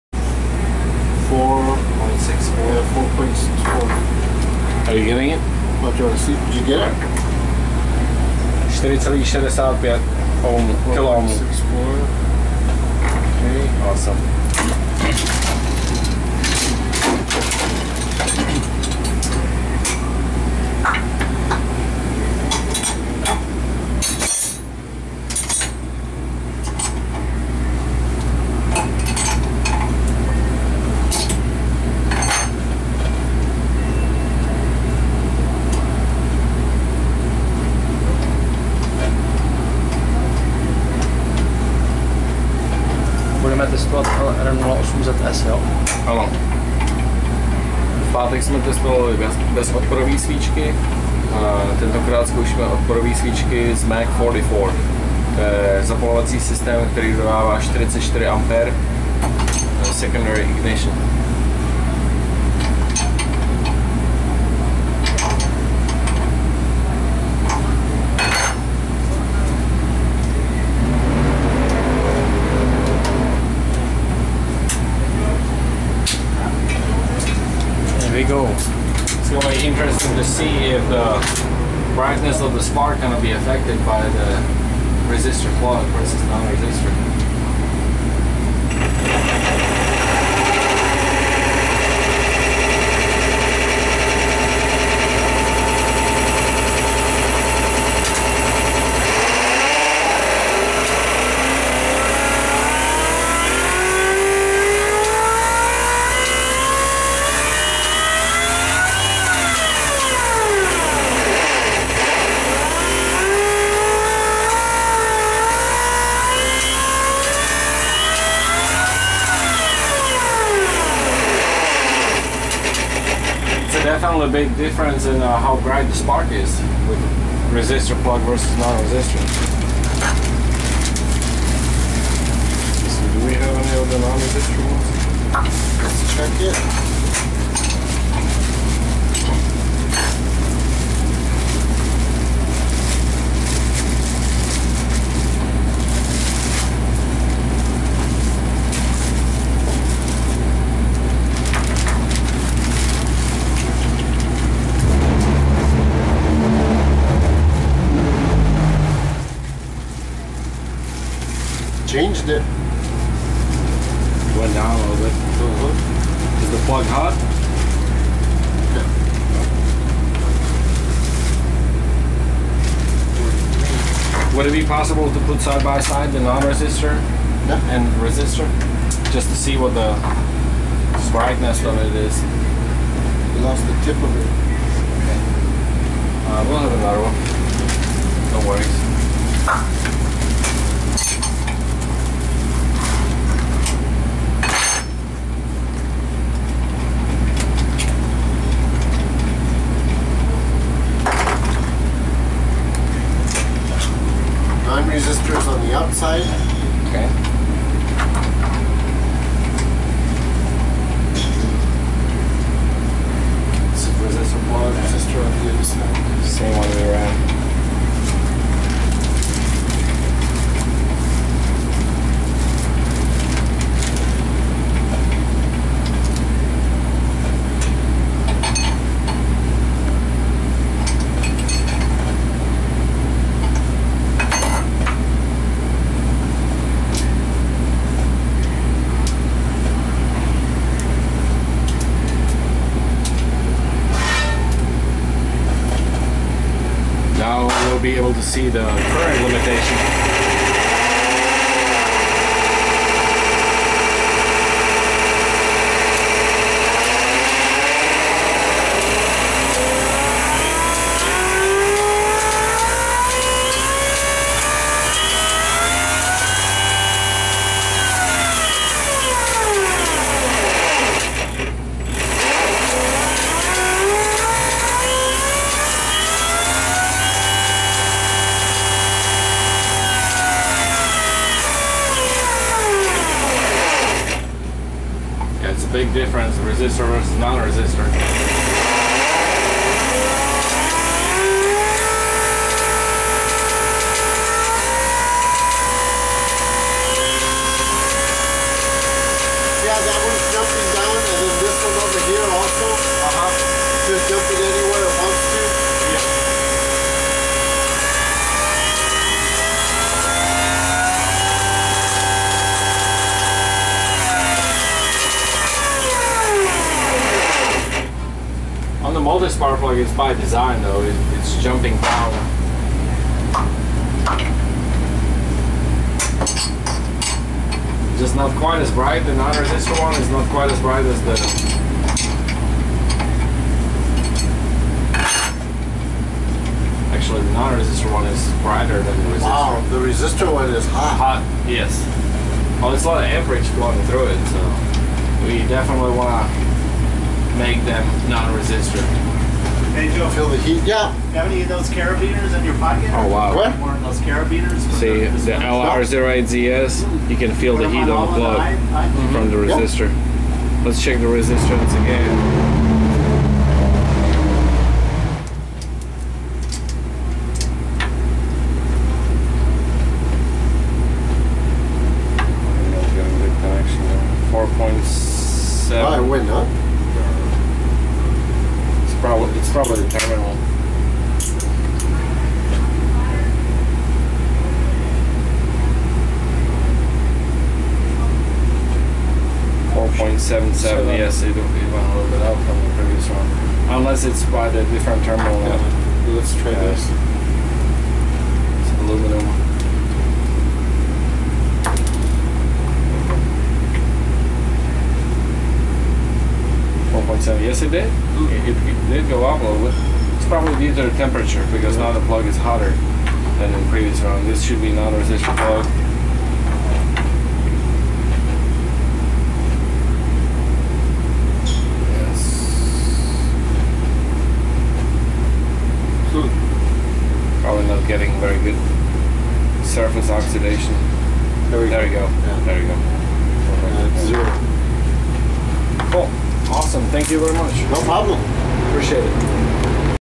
High Performance Brisk Premium Multi-Spark plug firing on MSD Mag 44
Comparison between Brisk Premium Multi-Spark plugs and NGK, Autolite and Bosch competitors!